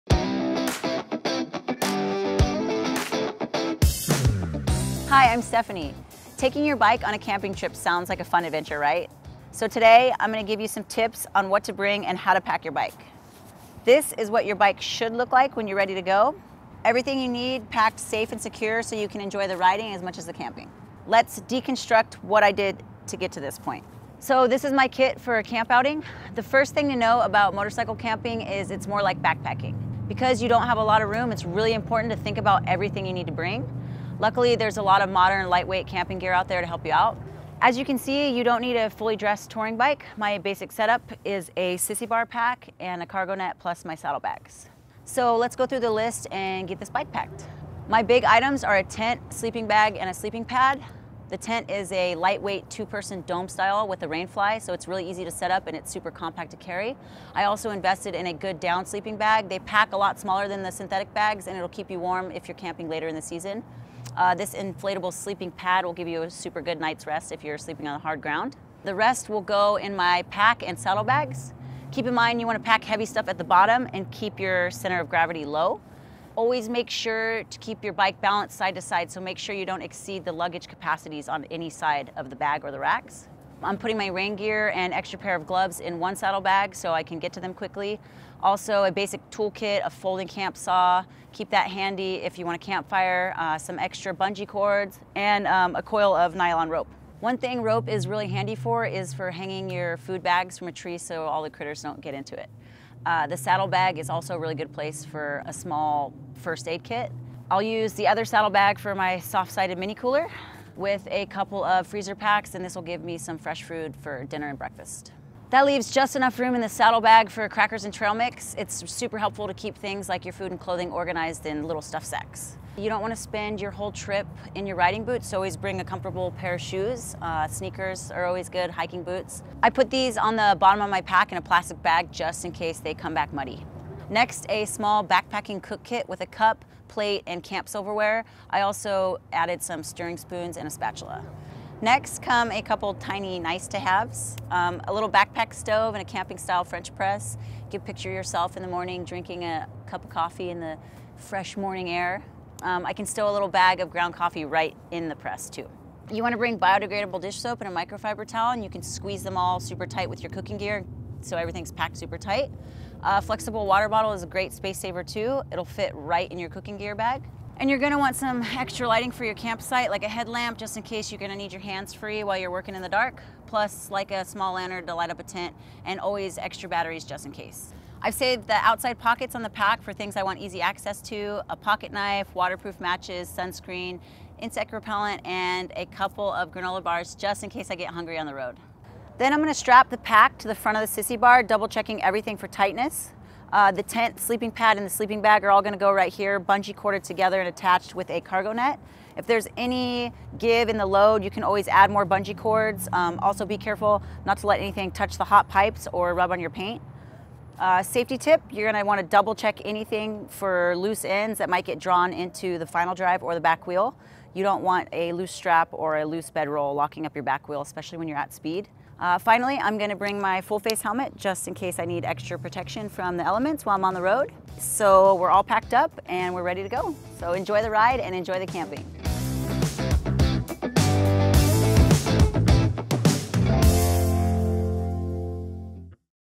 How To Pack for a Motorcycle Trip | Harley-Davidson Riding Academy
Tips on what camping gear to bring and how to pack your bike for an overnight motorcycle camping adventure.

Ready to learn more? Harley-Davidson Riding Academy courses are designed for riders of all skill levels, from beginner to advanced.